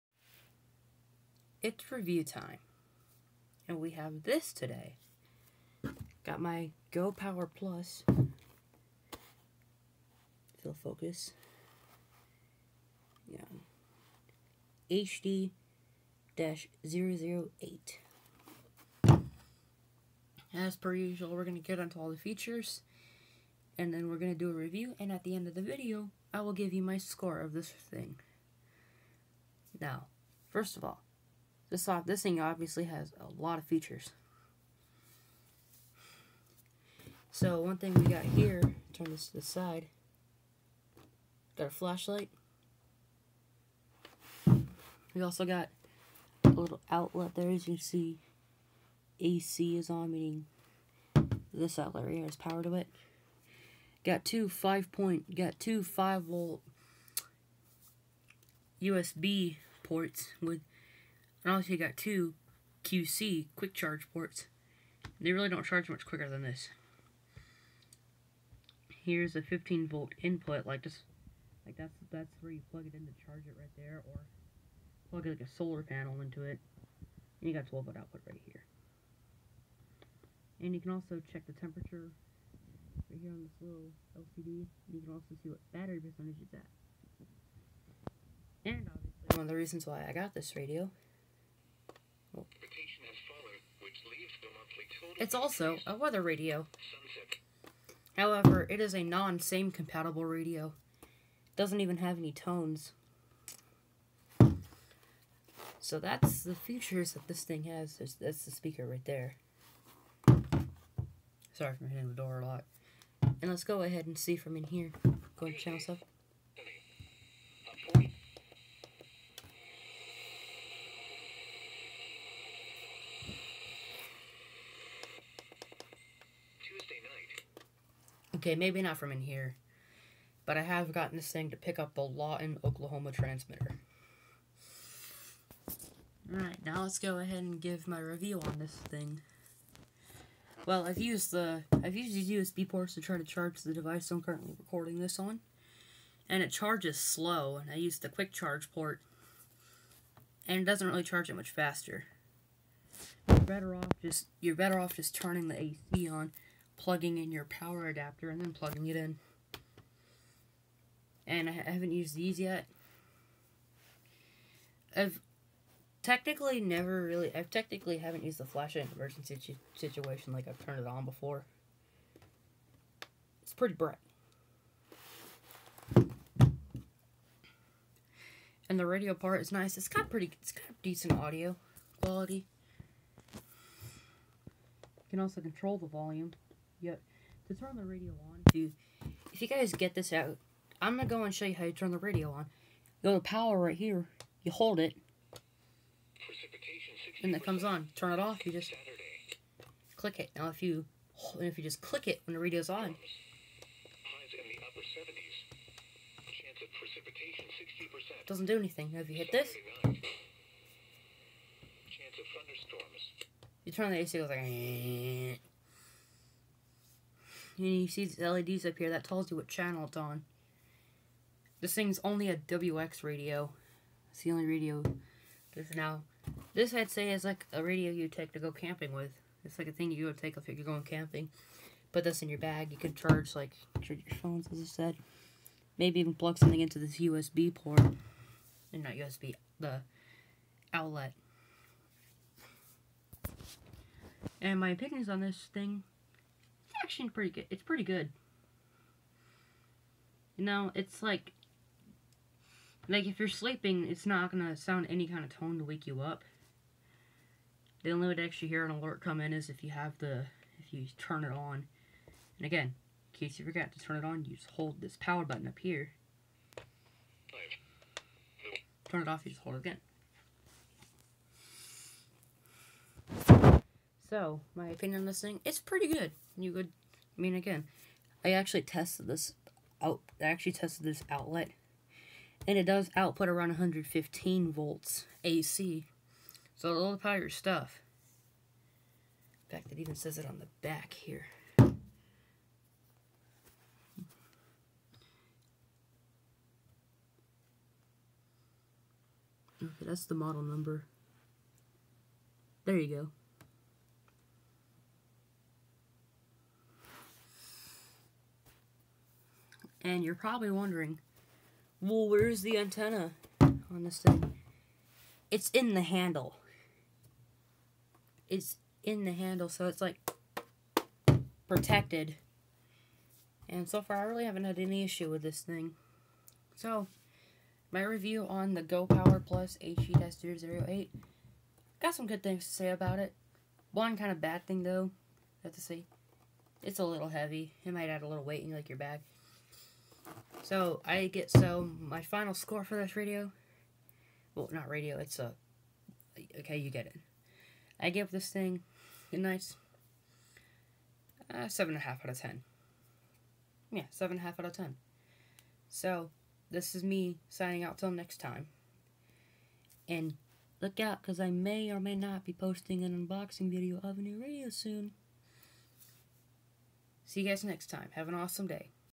GoPower Plus HD-008 Reveiw
In this video, I go over all the features, my Reveiw, my opinion on this, and even my final score, and this is perfect for camping, or if you want a radio to just listen to the weather, but as a said, it is just a NOAA Radio, so no Warning Alarms to wake you up, but with all that, enjoy